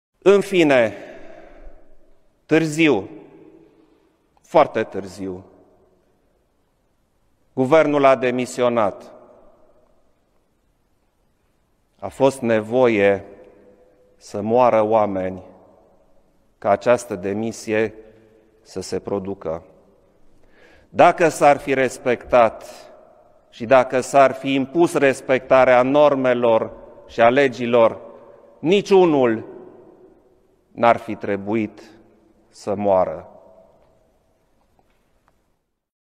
Iohannis 2015 Colectiv
2015: "A fost nevoie sa moara oameni ca aceasta demisie sa se produca. Daca s-ar fi impus respectarea normelor niciunul nu ar fi trebuit sa moara."
Daca s-ar fi impus respectarea normelor si in spitale atunci nimeni n-ar fi murit nici la Piatra Neamt